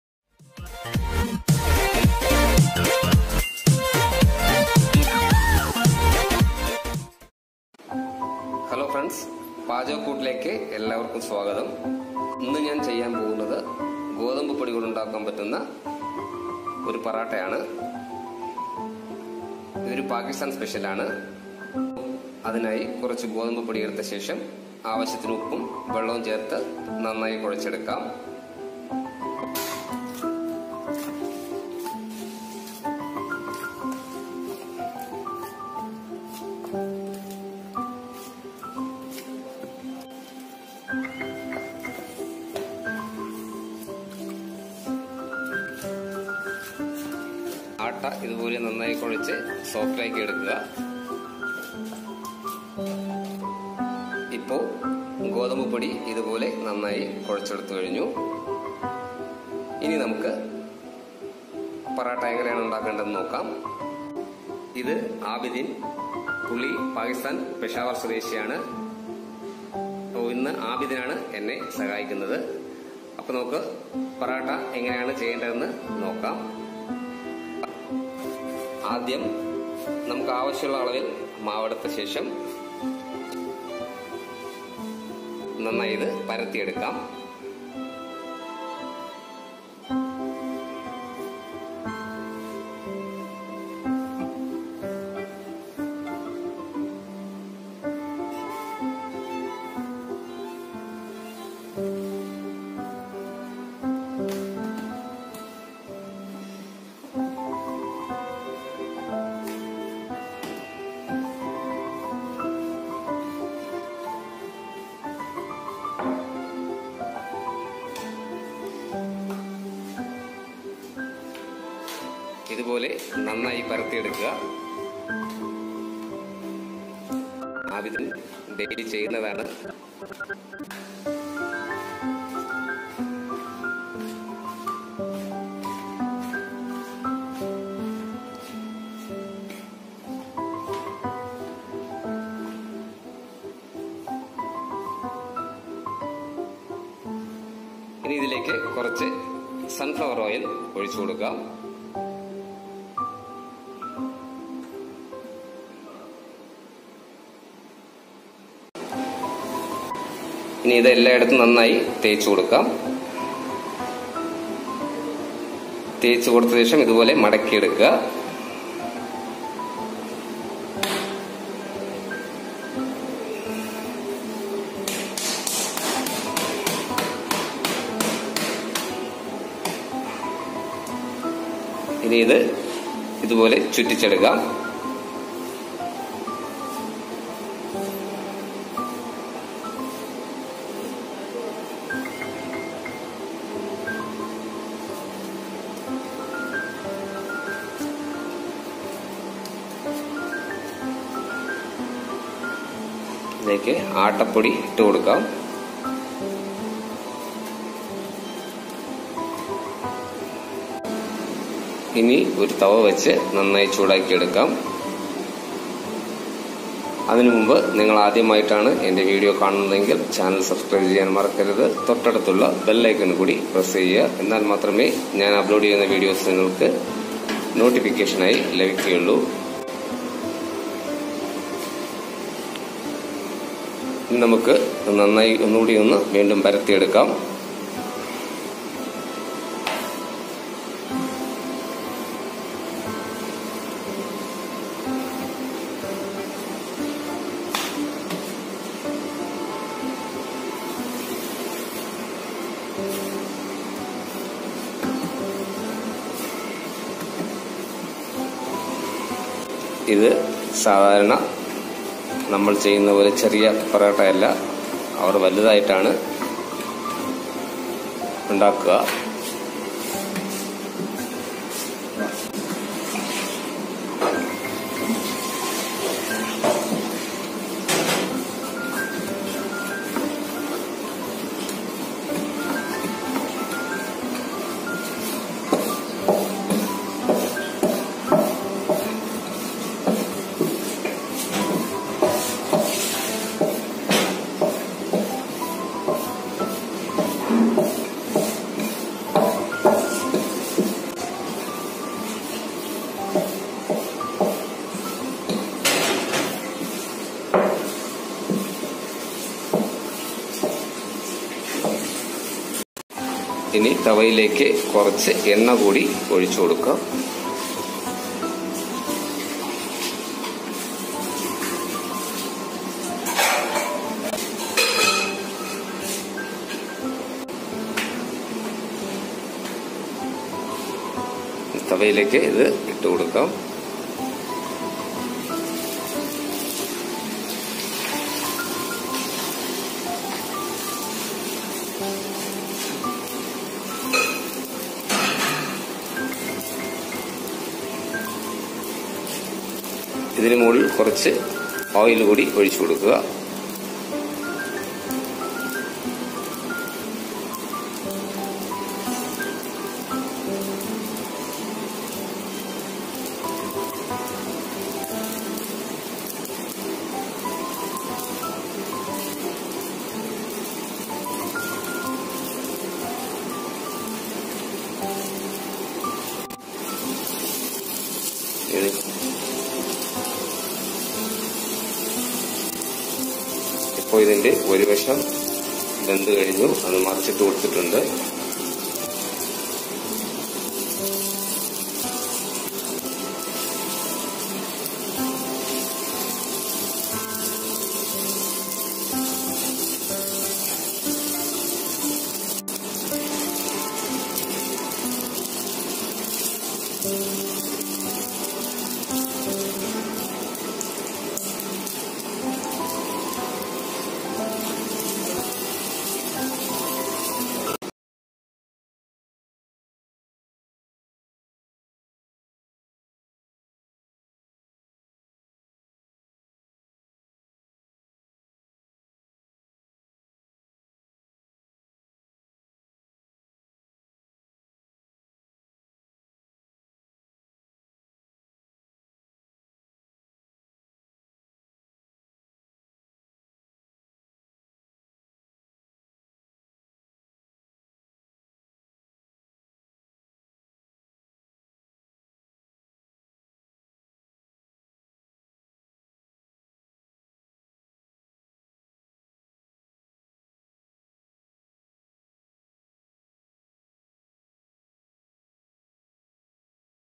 Pakistani Style Wheat Parotta | Pachaka koottu | Pravasi special | Video 31
Wheat flour
Sunflower oil
Salt
Water